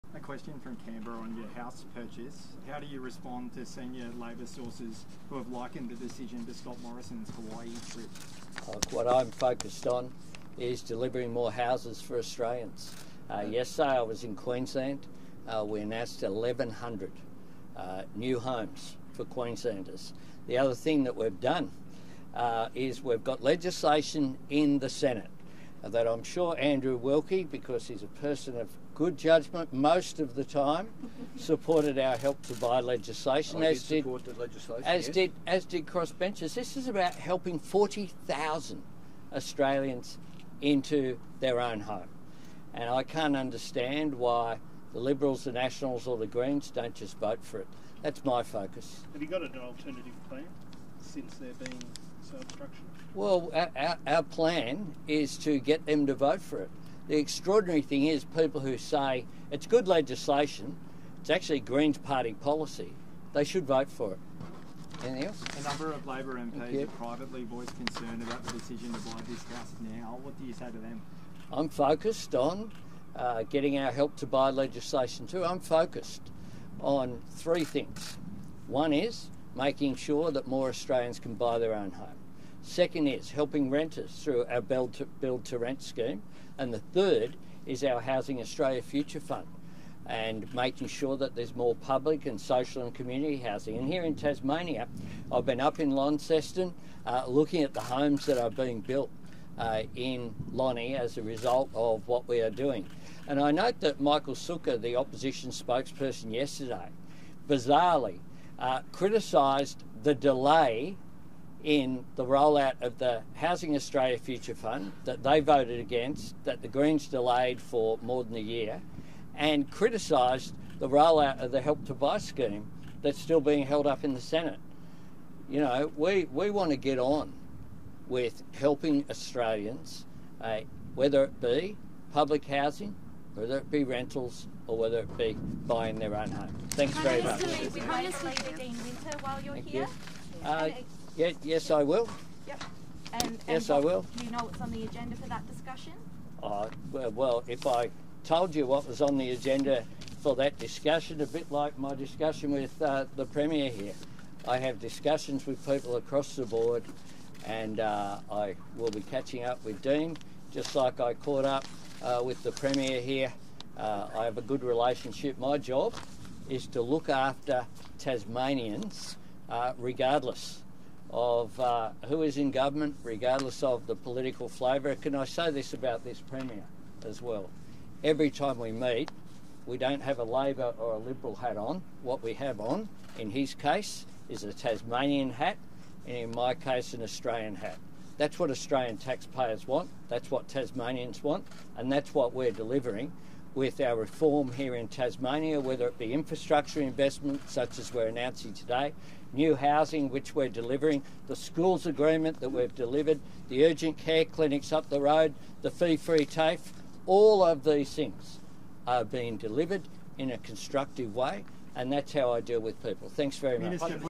PM pressed on $4.3 million property purchase as comparison drawn with Scott Morrison’s Hawaii trip
Prime Minister Anthony Albanese has been pressed on his recent purchase of a multi-million-dollar clifftop home in a popular beachside town during a press conference on Wednesday.

One reporter referenced a comment by a senior Labor source, as reported by the ABC, which compared the Prime Minister’s recent purchase to Scott Morrison’s trip to Hawaii during the 2019 Black Summer bushfires.

“What I am focused on is delivering more houses for Australians,” the Prime Minister responded.